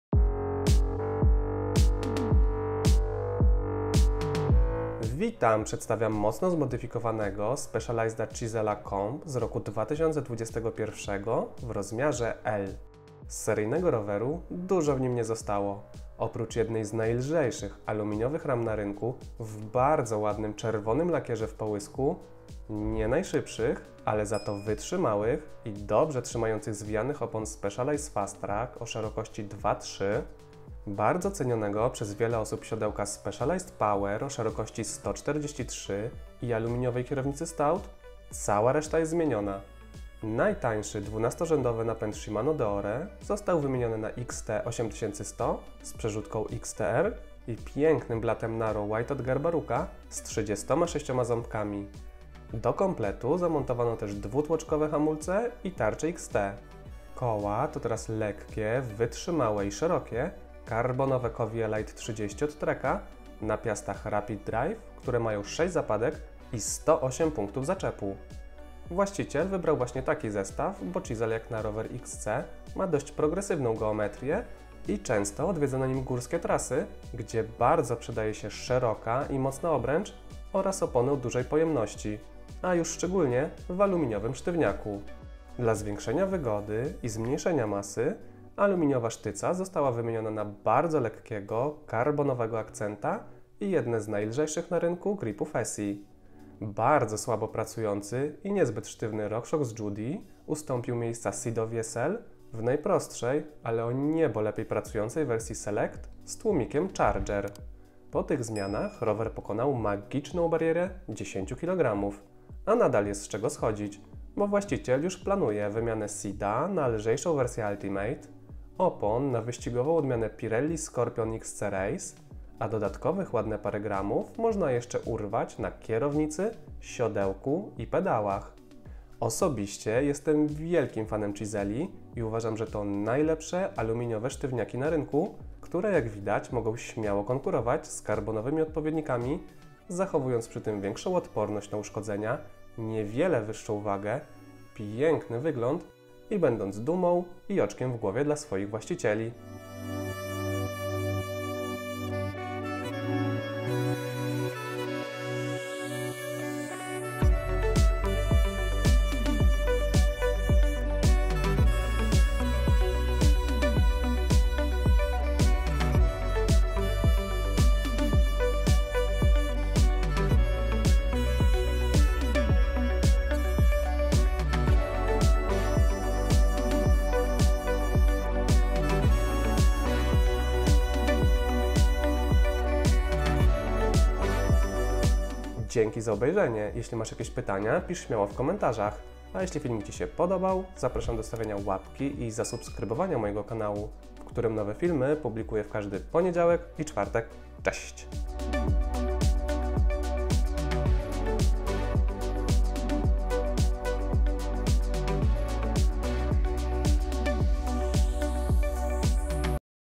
SPECIALIZED CHISEL Comp 2021 // Duże Modyfikacje i Waga Poniżej 10Kg
Przedstawiam mocno zmodyfikowanego Specialized'a Chisel'a Comp z roku 2021, w rozmiarze L.

👉 Z A P R A S Z A M D O O B E J R Z E N I A